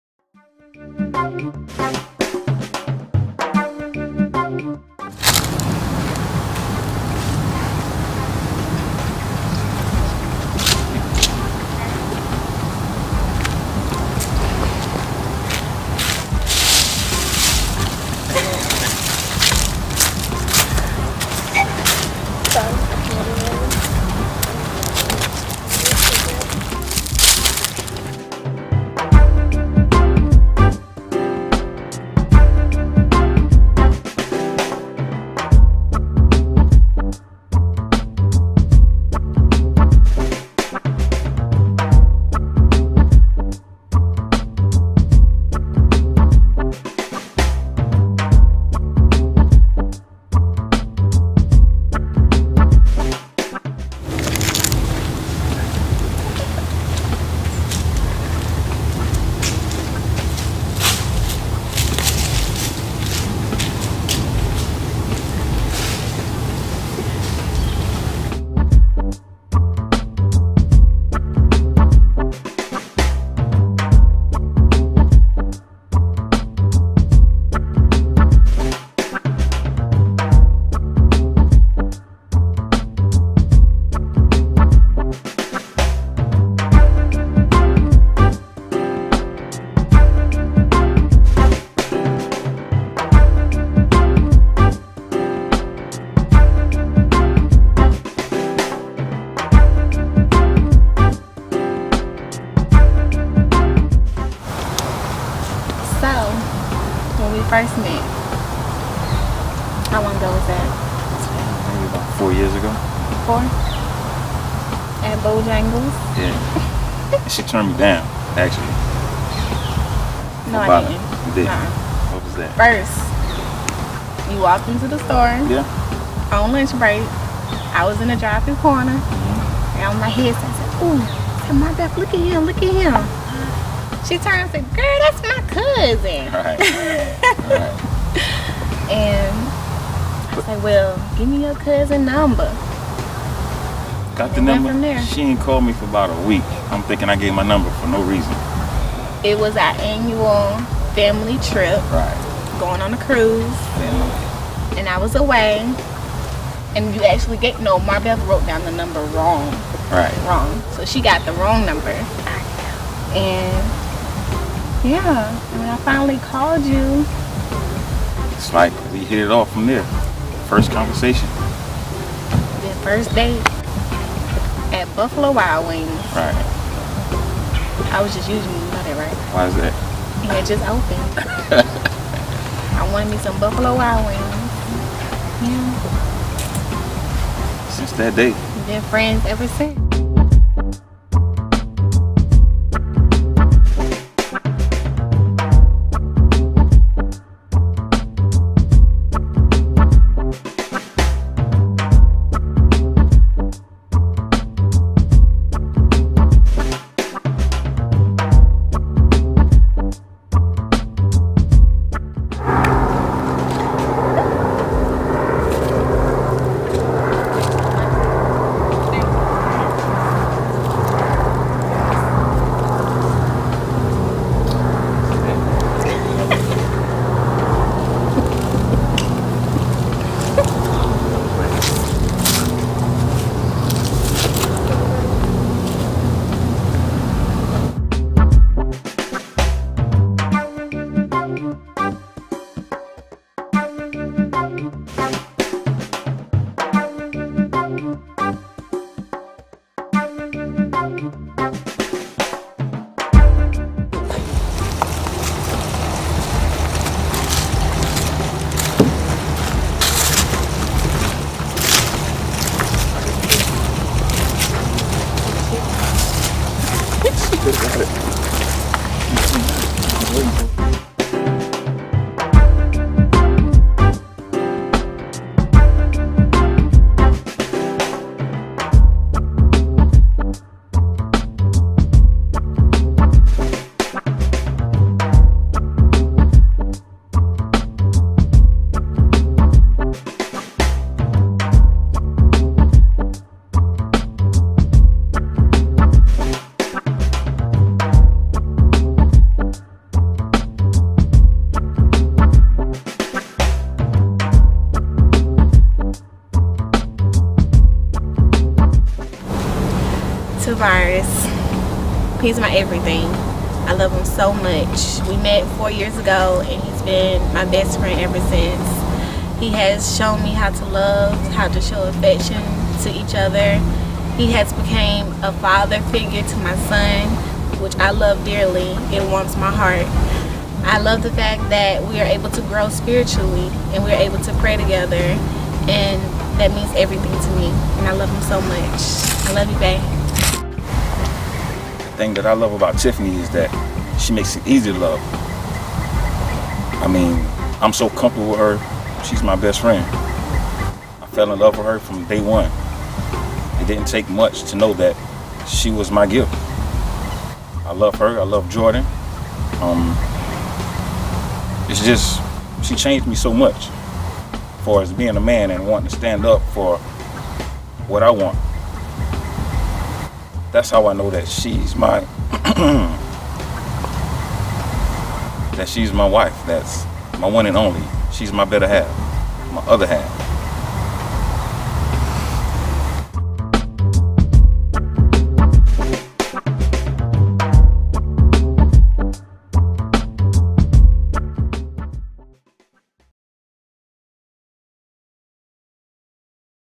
Tiphanie & Tavares Engagement Session
Tiphanie and Tavares, a young couple in love. They wanted to do the engagement session at a park that had be devastated by the hurricane that came through months earlier. The love that they had between them was no match for the debris and down trees. We walked and talked as if it was just another sunny day, and I was there to capture it all.